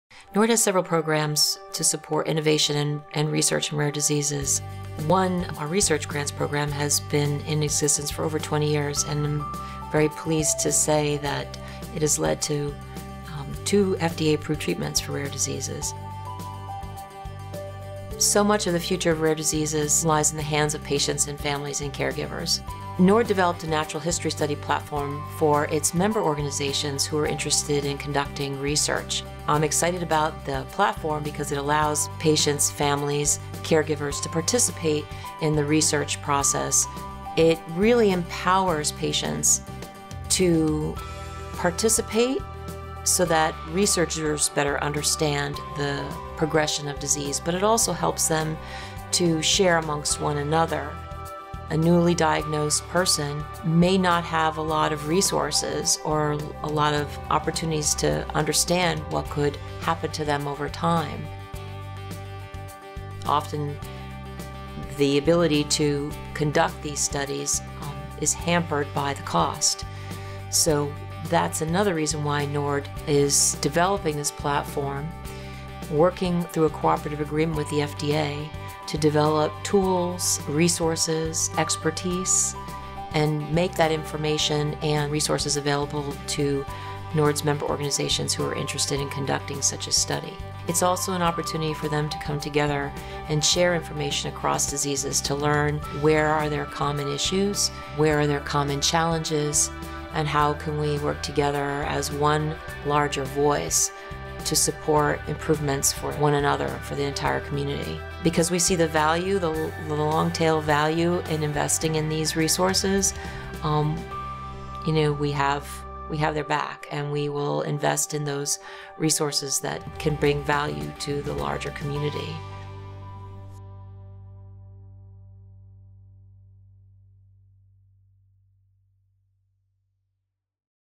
Research; Making a Rare Impact
NORD's Research Programs support innovation and research in rare diseases and have led to two FDA approved treatments for rare diseases, making a Rare Impact.